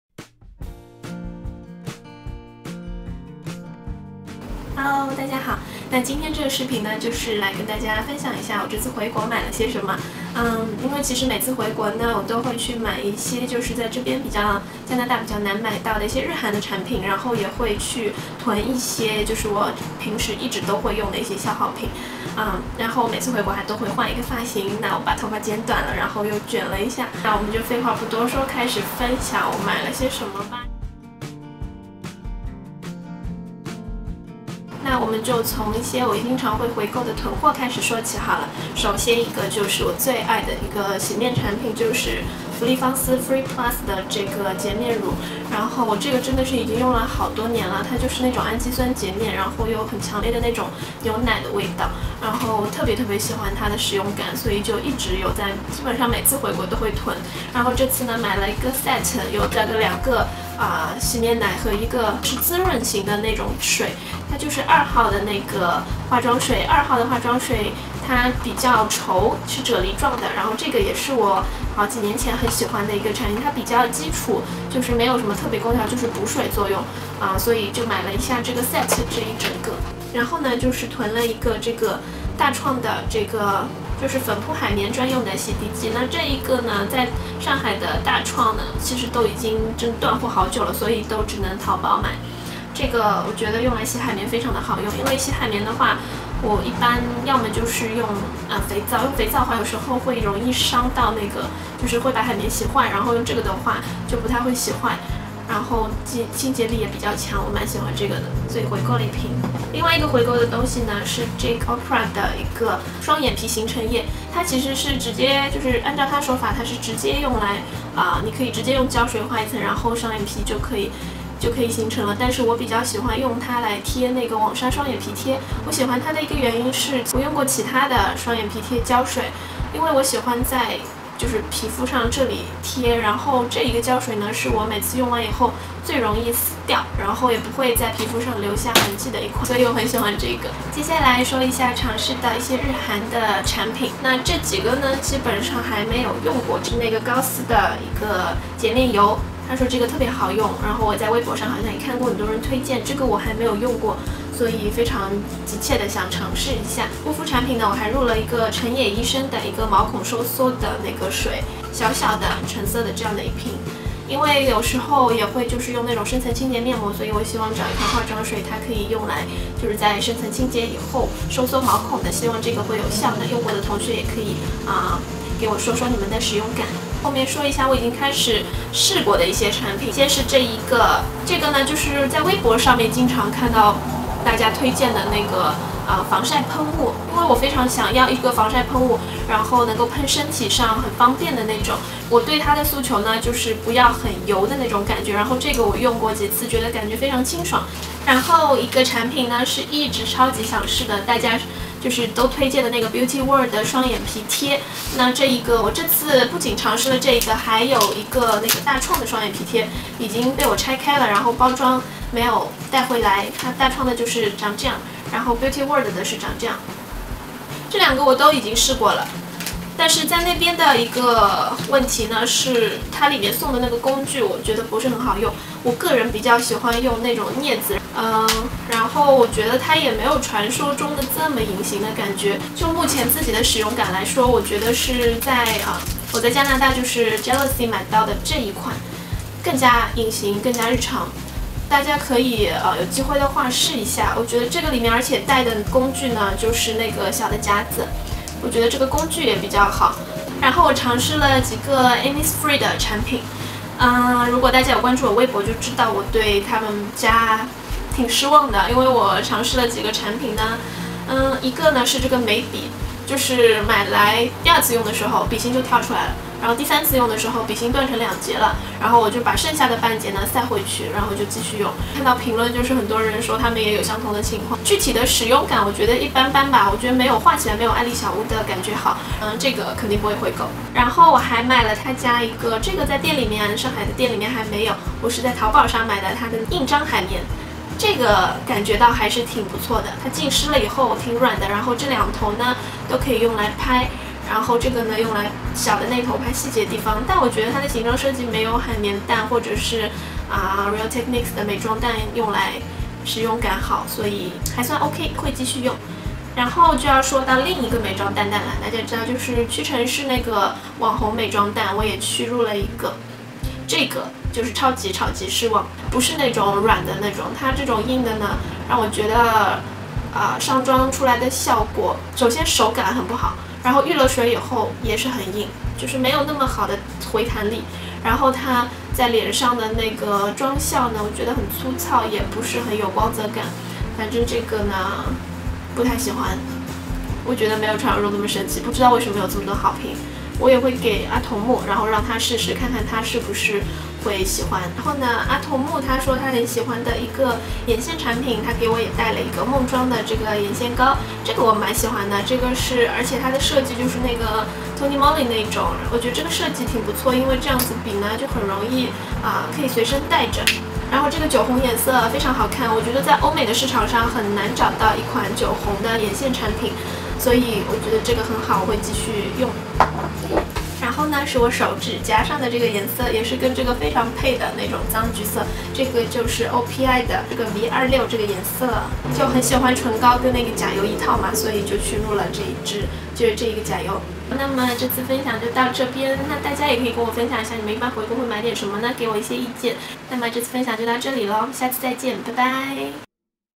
Sherryi77 | Beauty Haul | Back to China 回国购物分享 September
❤Hello 大家好!九月初在国内购物分享带给大家，来看看我回国一般都会买些什么吧～
也欢迎大家和我分享你们的购物清单～～

❤Other Video you might be interested:
网纱双眼皮贴佩戴方法